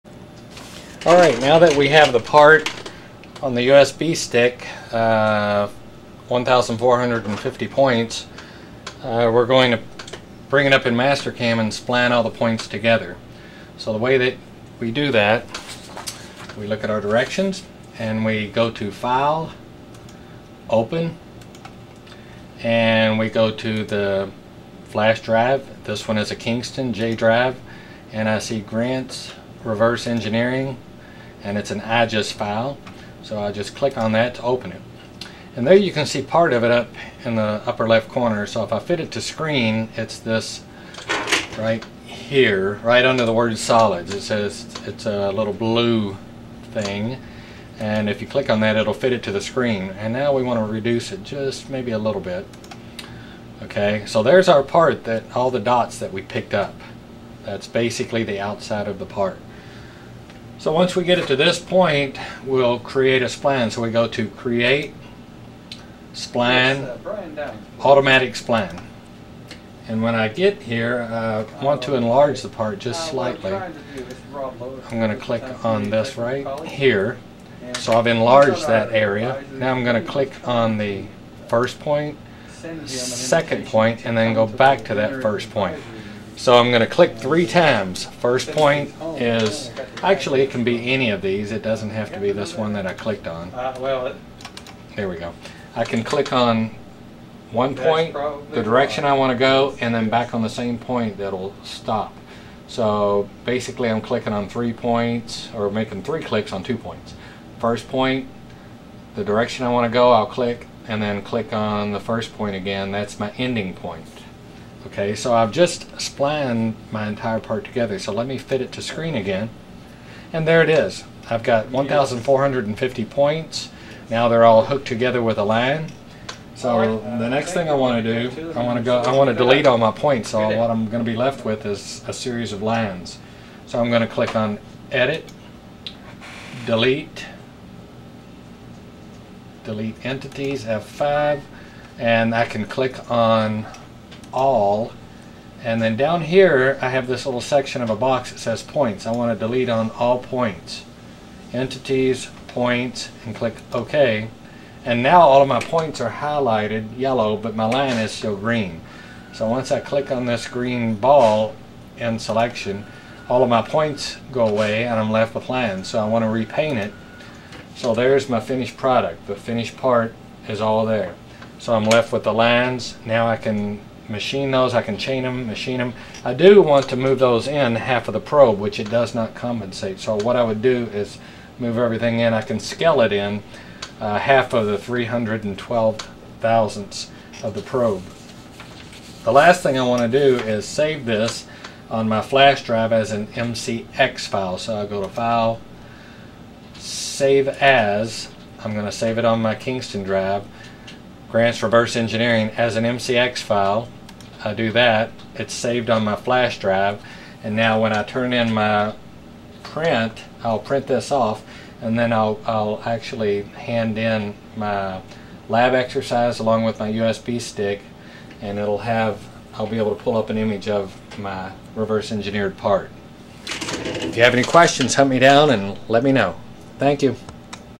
Reverse engineering using the MasterCam software
This video will show you how to use MasterCam software to create a splined project.